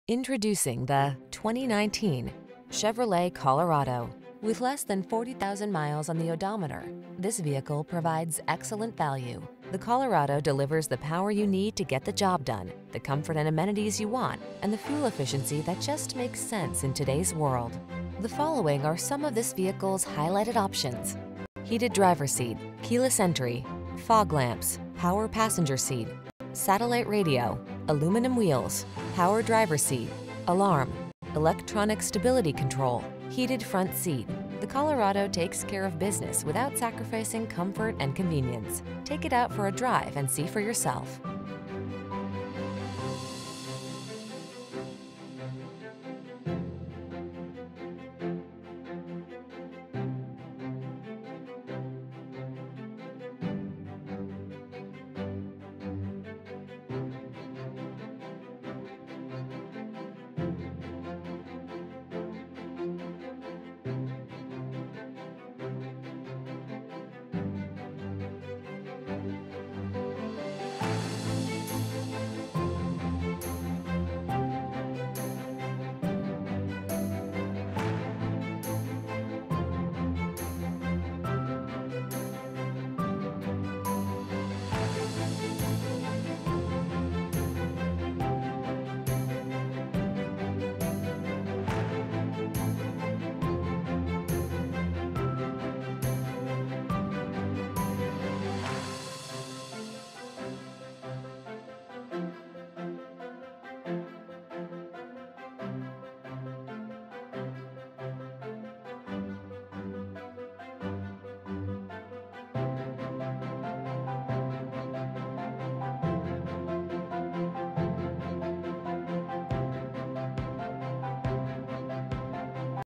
2019 Chevrolet Colorado Muncie, Anderson, Kokomo, Marian, Indianapolis 220652A
Summit White Used 2019 Chevrolet Colorado Z71 available in 6580 W. Hometown Blvd., Muncle, Indiana at All American Chevrolet Cadillac. Servicing the Muncie, Anderson, Kokomo, Marian, Indianapolis, IN area.

Colorado Z71
2019 Chevrolet Colorado Z71 - Stock#: 220652A

All American Chevrolet Cadillac
6580 W Hometown Blvd
Muncie IN 47304